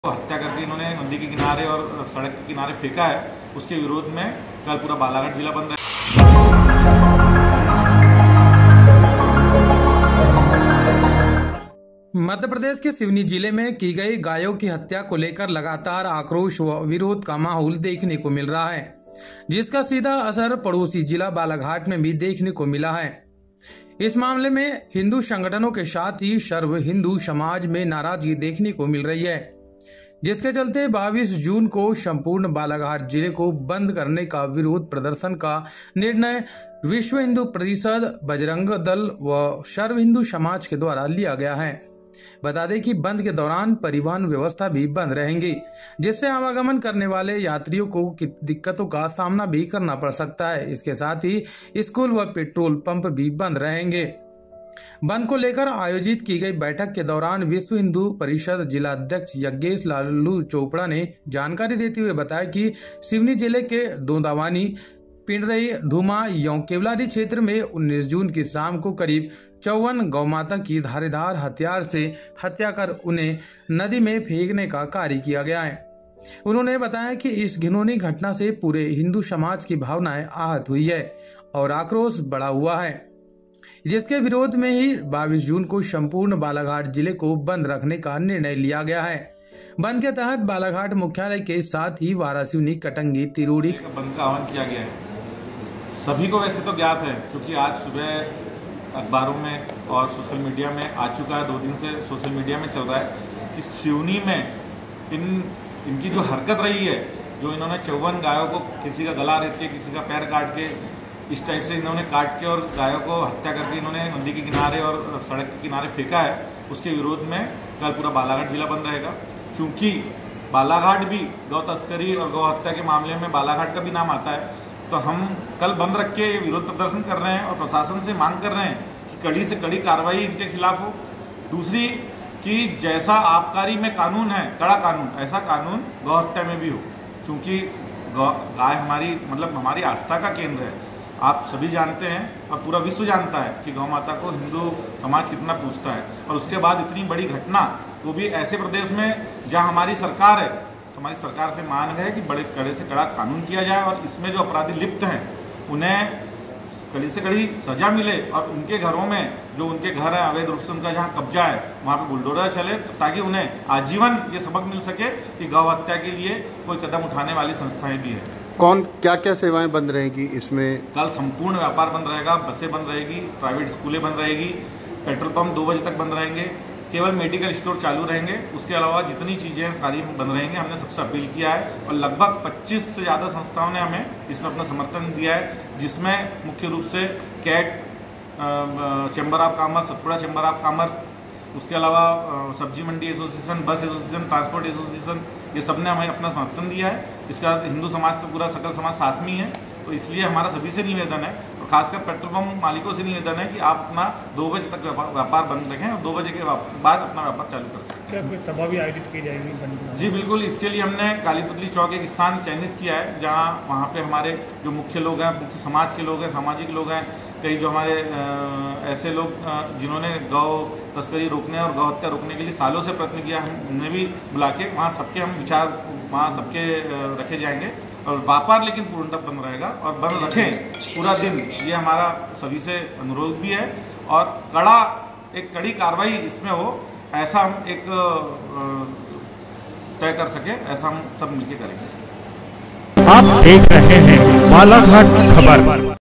BGT khabar: बंद..बंद..बंद, बालाघाट बंद..! | सिवनी गौहत्या कांड की लौ..BGT | Seoni cow slaughter case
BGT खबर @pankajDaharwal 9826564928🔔

बालाघाट..बंद..बंद..बंद
सिवनी गौहत्या कांड की लौ..
सिवनी गौहत्या कांड को लेकर आज जिला बंद

मध्यप्रदेश के सिवनी जिले में की गई गायों की हत्या को लेकर लगातार आक्रोश व विरोध का माहौल देखने को मिल रहा है। जिसका सीधा असर पड़ोसी जिला बालाघाट में भी देखने को मिल रहा है। इस मामले से हिंदू संगठनों के साथ ही सर्वहिंदू समाज में नाराजगी देखने को मिल रही है। जिसके चलते ही 22 जून को संपूर्ण बालाघाट जिले को बंद कर विरोध करने का निर्णय विश्व हिंदू परिषद, बजरंग व सर्व हिंदू समाज के द्वारा लिया गया है। बता दें कि बंद के दौरान परिवहन व्यवस्था भी बंद रहेगी जिससे आवागमन करने वाले यात्रयों को दिक्कतों का सामना भी करना पड़ सकता हंै। इसके साथ स्कूल व पेट्रोल पंप भी बंद रहेंगे।
54 गायों को काटकर की गई हत्या
बंद को लेकर आयोजित की गई बैठक के दौरान विश्व हिंदू परिषद जिलाध्यक्ष यज्ञेश लालू चावड़ा ने जानकारी देते हुए बताया कि सिवनी जिले के दोंदावानी, पिंडरई,धूमा एवं केवलारी क्षेत्र में 19 जून की शाम को करीब 54 गौमाता की धारदार हथियार से हत्या कर उन्हें नदी में फेंकने का कार्य किया गया है। उन्होंने बताया कि इस घिनौनी घटना से पूरे हिंदू समाज की भावना आहत हुई है और आक्रोश बढ़ा हुआ हैं। जिसके विरोध में ही 22 जून को संपूर्ण बालाघाट जिले को बंद रखने का निर्णय लिया गया है। बंद के तहत बालाघाट मुख्यालय के साथ ही वारासिवनी, कटंगी, तिरोड़ी, खैरलांजी, लालबर्रा, बैहर, बिरसा, परसवाड़ा, लांजी सहित अन्य तहसील व ग्रामीण क्षेत्र भी बंद रहेंगे।